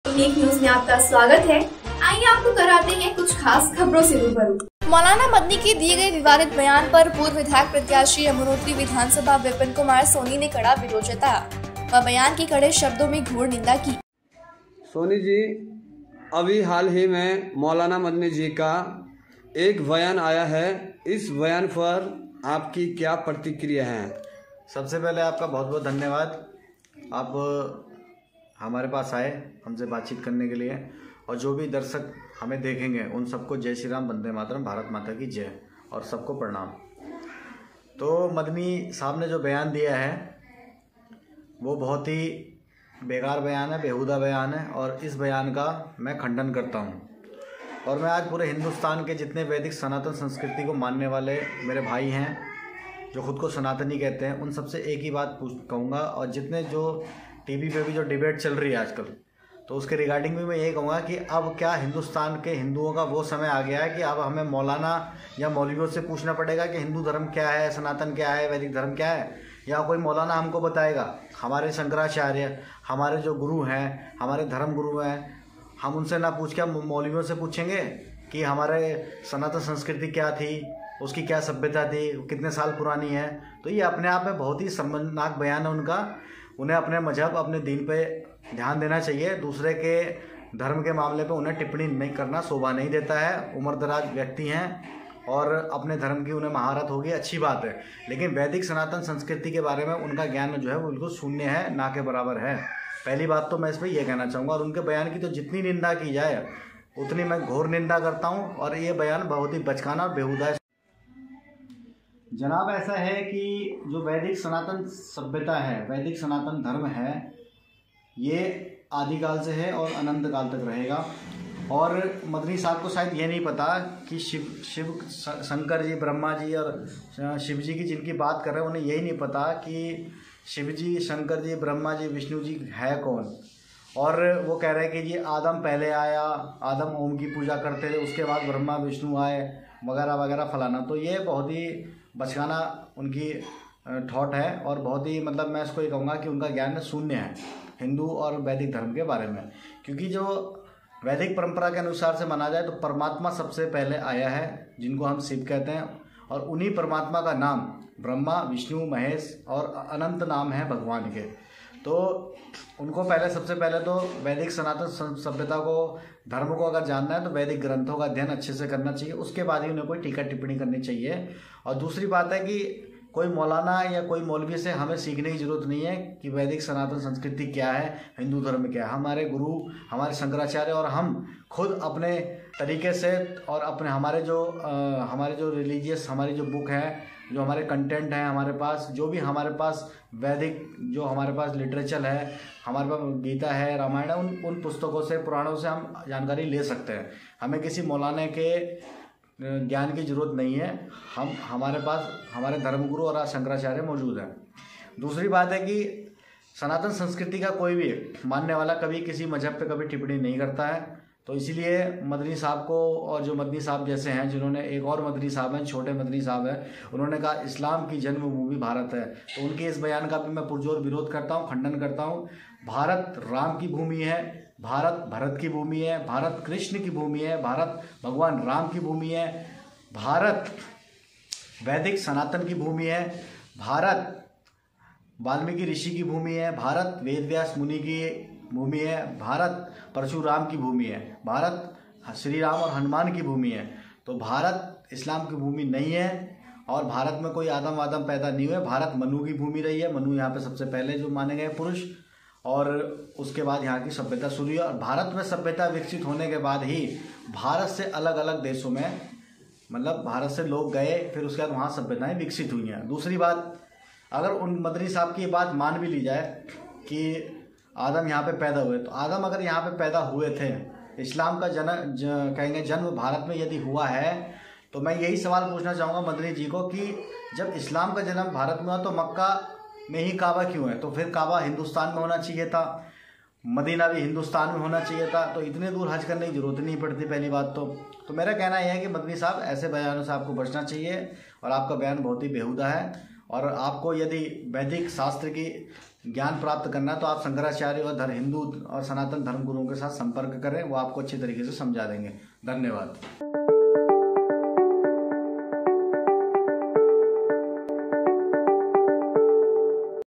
यमुनोत्री: मौलाना मदनी के दिए गए विवादित बयान पर विपिन कुमार सोनी ने जताया कड़ा विरोध
j.p pahadi ki report